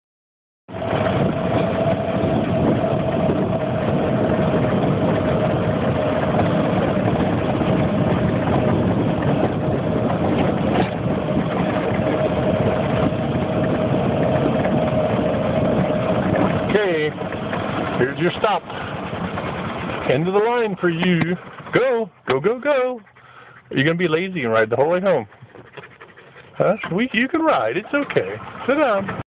Maggie's tongue says "I'm thirsty"!
Taking Maggie home for a drink of water.